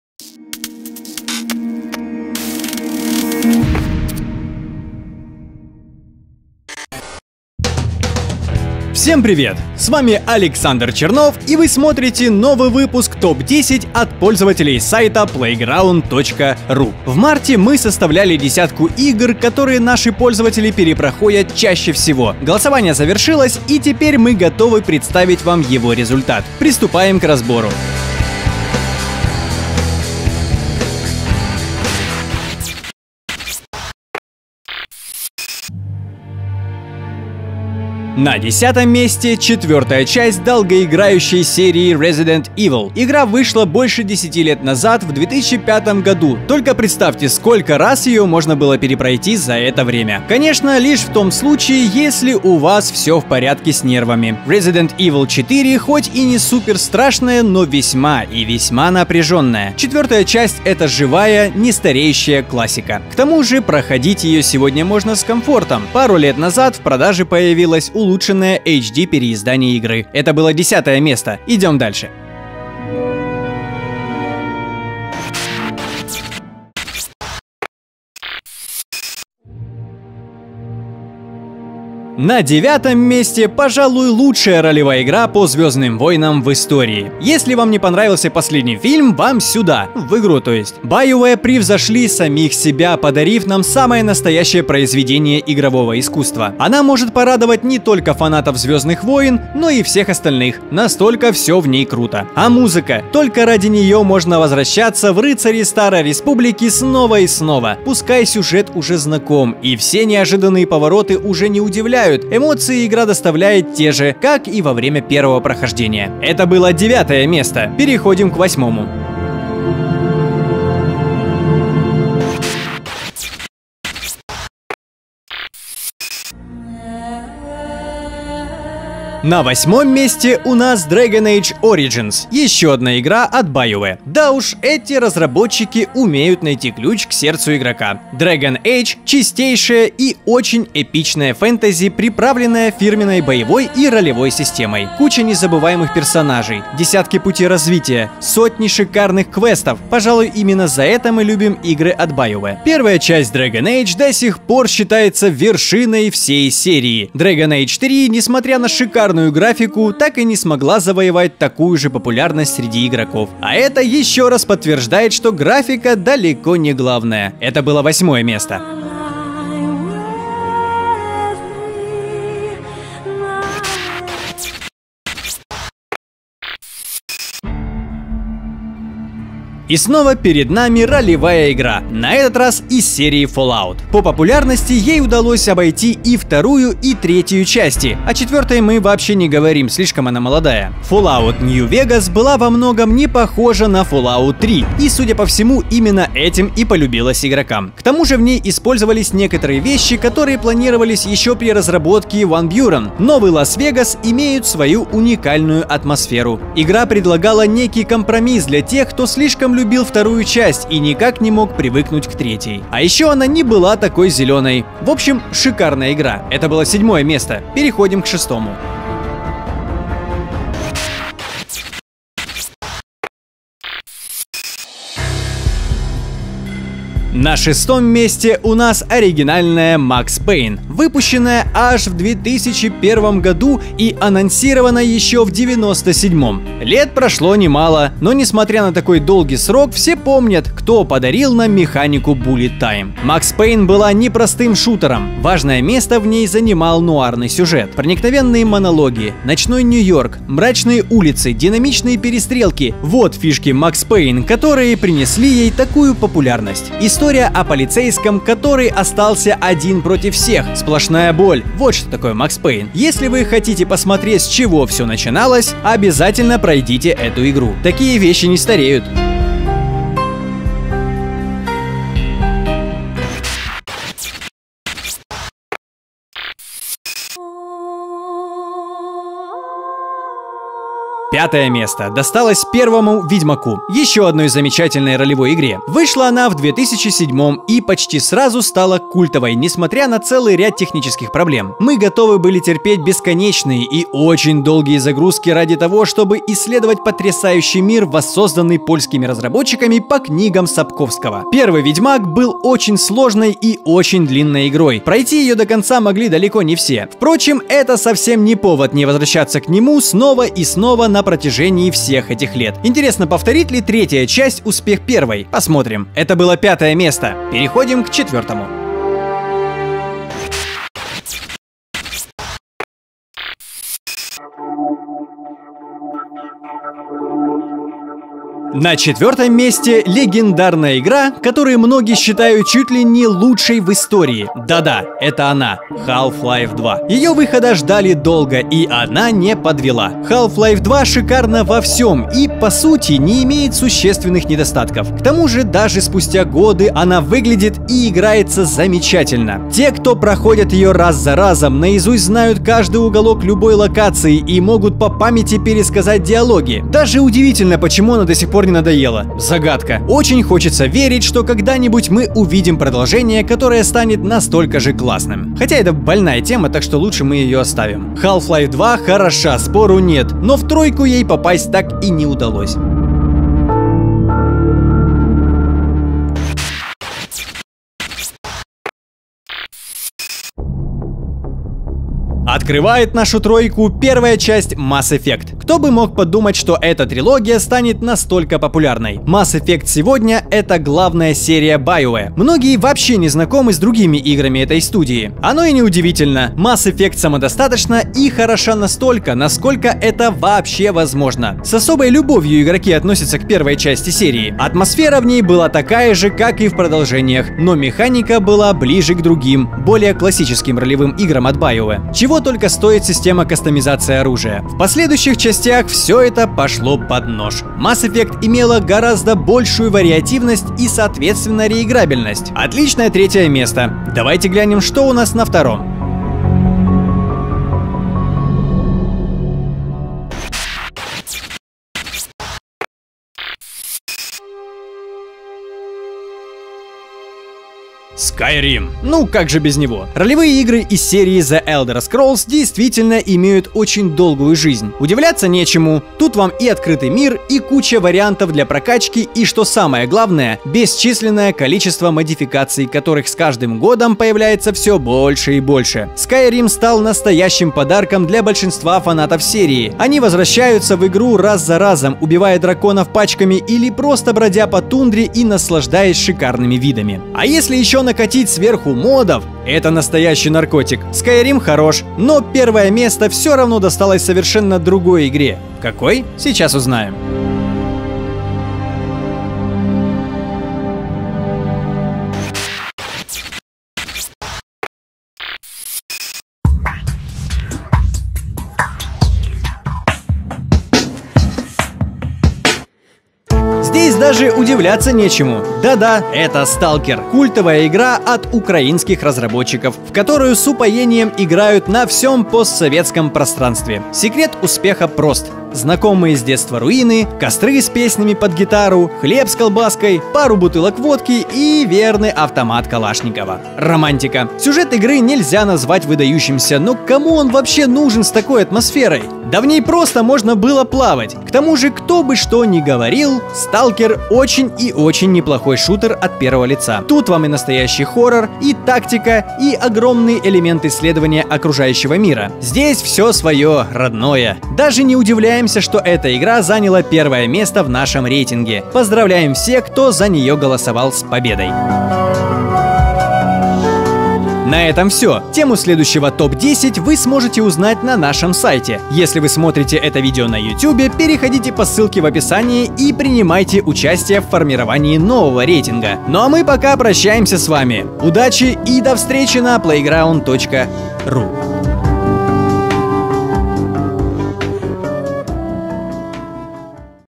ТОП-10: игры, которые вы перепроходите чаще всего
Игры, которые чаще всего перепроходят пользователи PlayGround.ru на основе голосования, проводившегося в марте 2016 года.